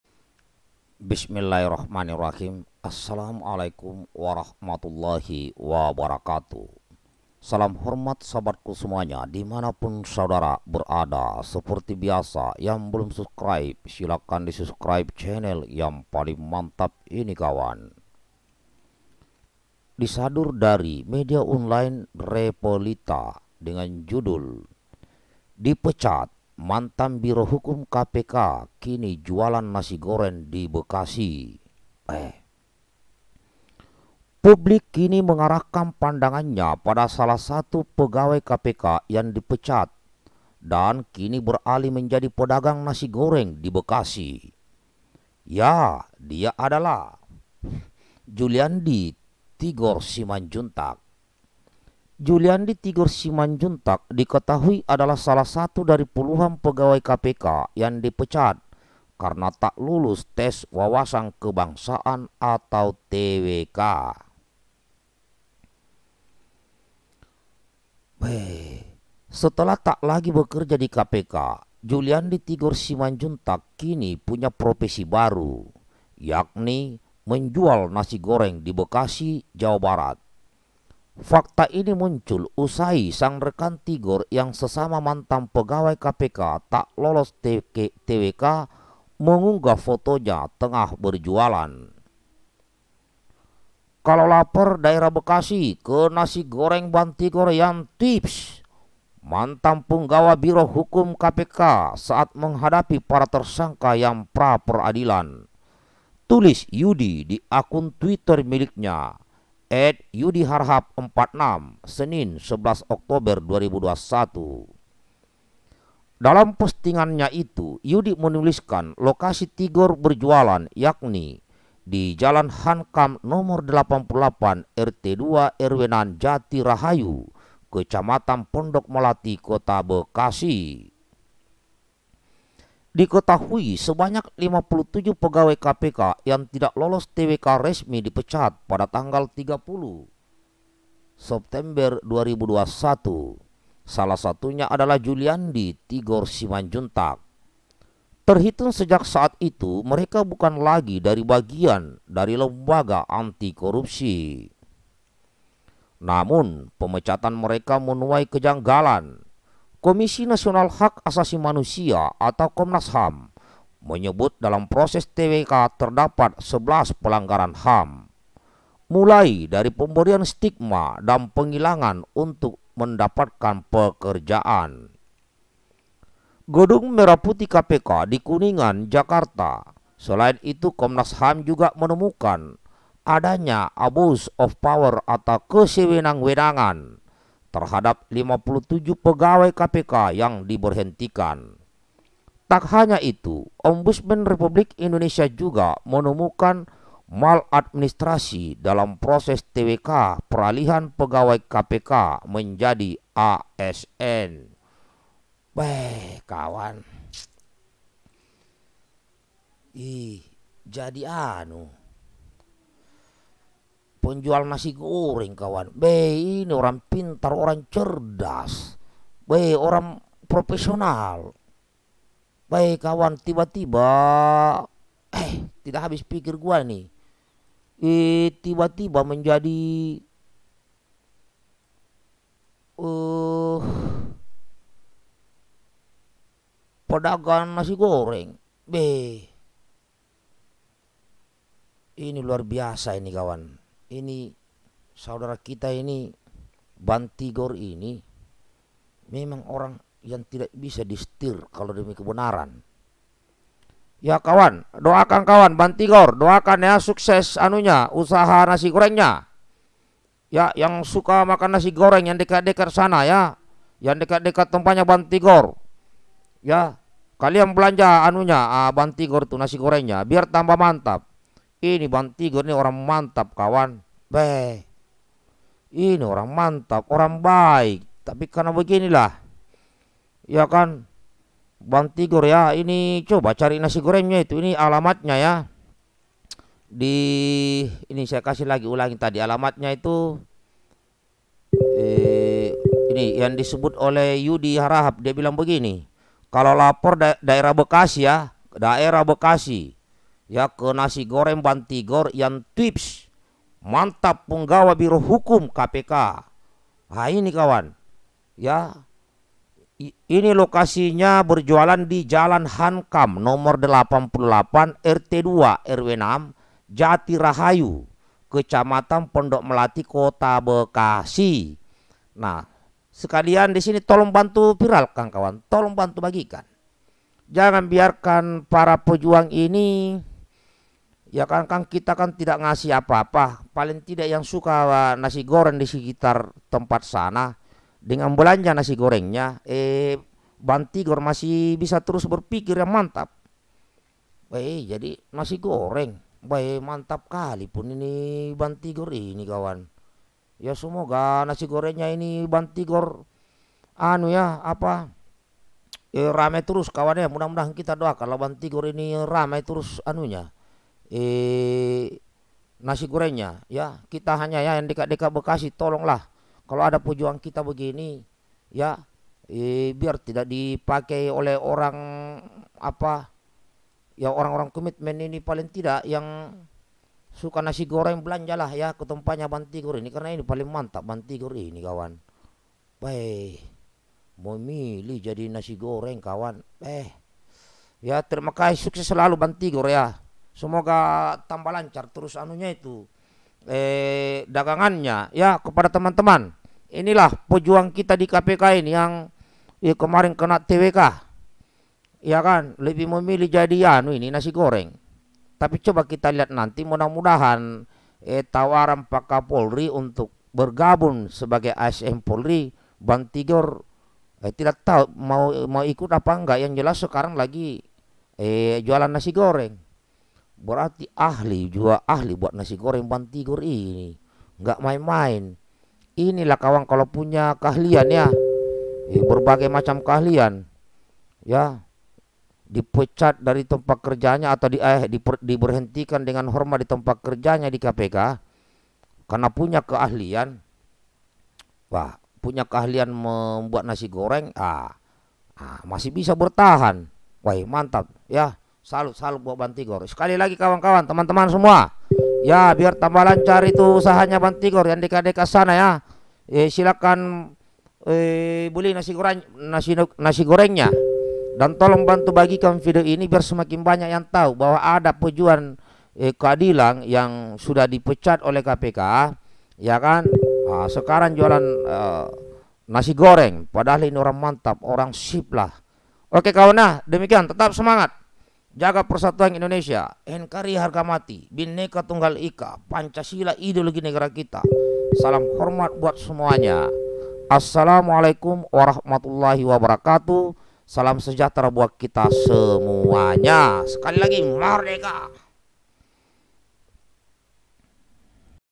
HEBOH. Setelah Dipecat kini mantan biro hukum KPK jualan nasi goreng di Bekasi. viralkan ya.
Aamiin.

TTD